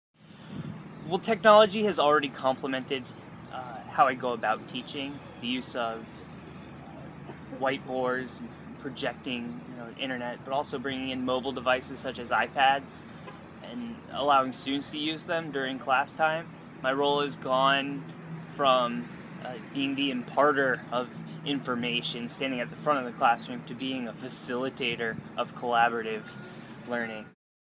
HGTC's MWI Interviews Dean Bloomberg pt2
Horry-Georgetown Technical College's Mobile Web Initiative, lead by Andre Pope, interviews faculty member Dean Bloomberg about his use of mobile technology in higher education.